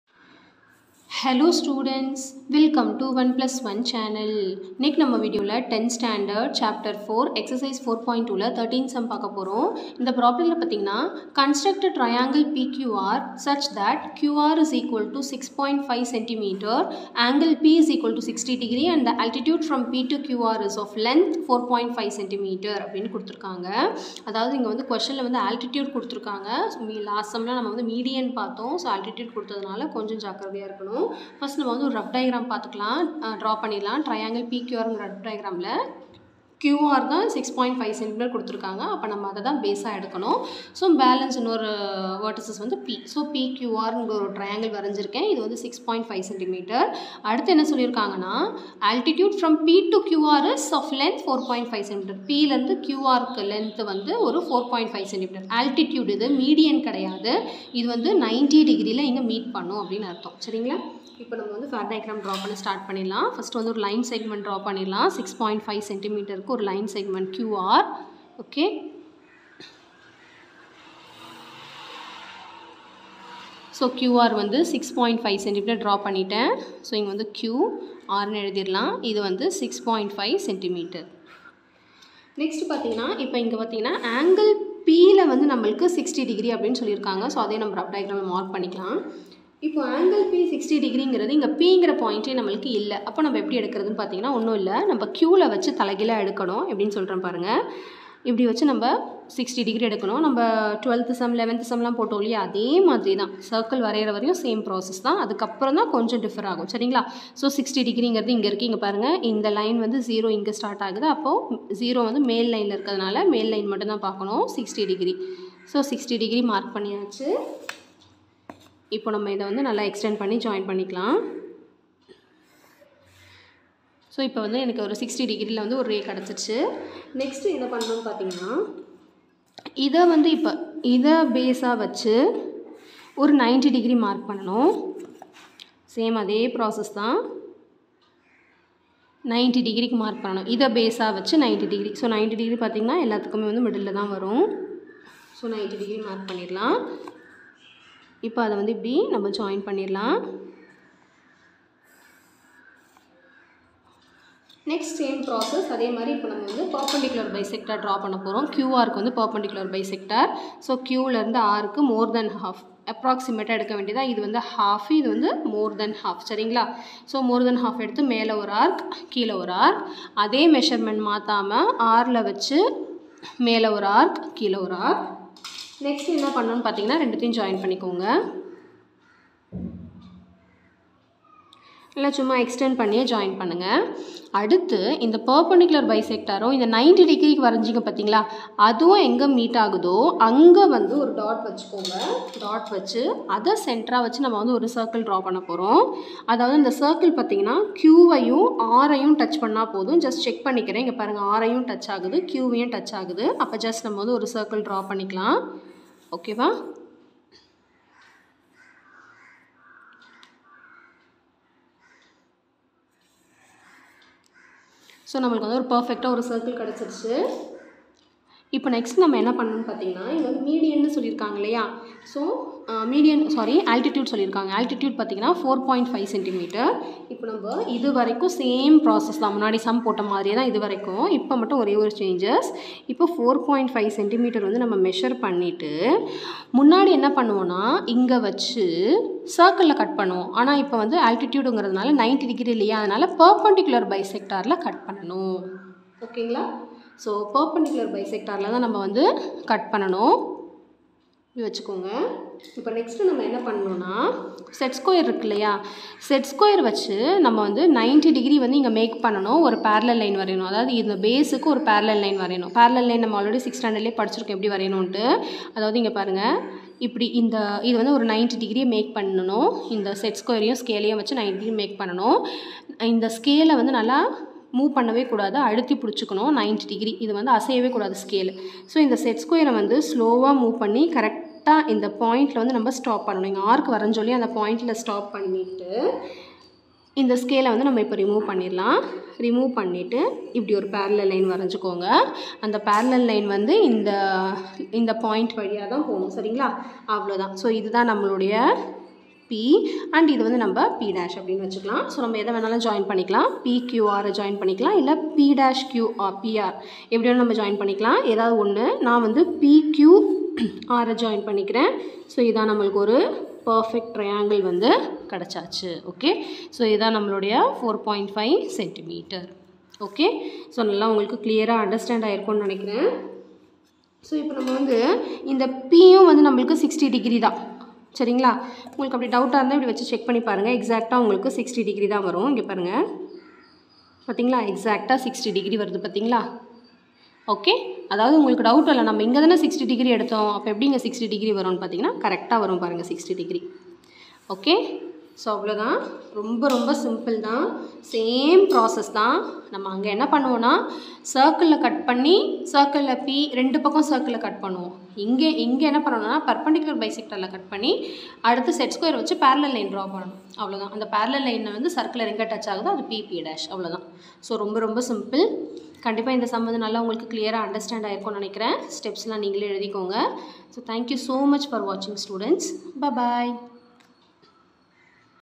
10th maths Ch-4 Geometry Ex-4.2 (13th sum) | Samacheer One plus One channel
This video contains for 10th standard maths Chapter-4 Geometry, Exercise-4.2 Q.No. 13 complete solution.

My Another Channel 👇
Sangeetha Media